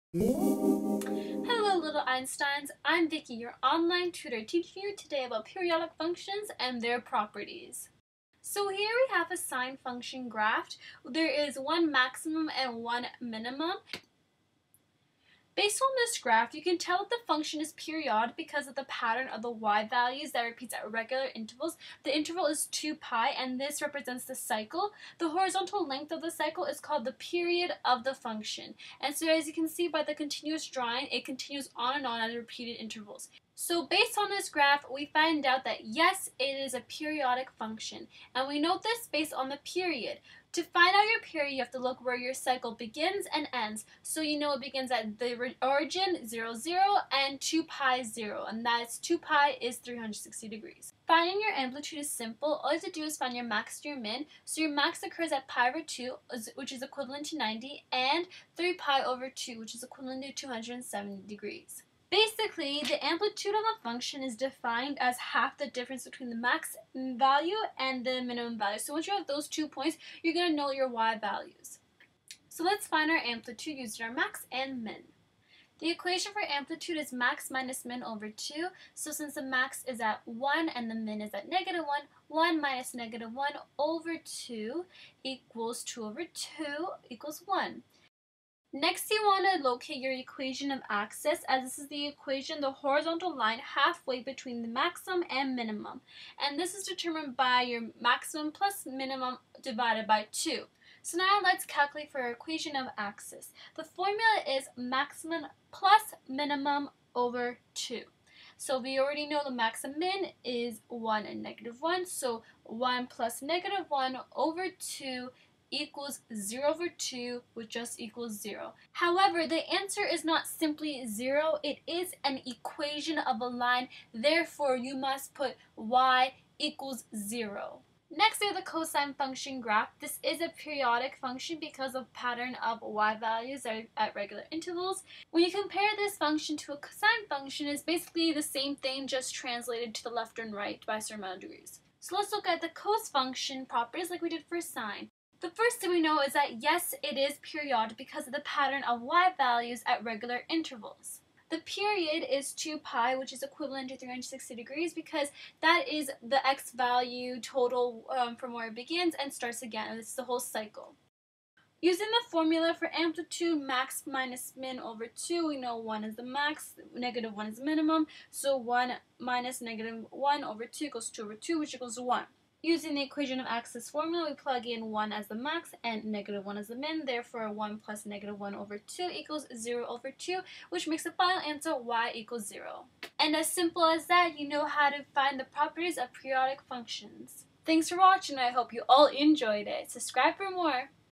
Let's Enjoy School: Periodic Functions and Their Properties
Hello Little Einsteins,

Let's ENJOY some math lessons! Today, we'll learn about periodic functions and their properties through period, max., min., amplitude, and equation of axis!

[LET'S TALK]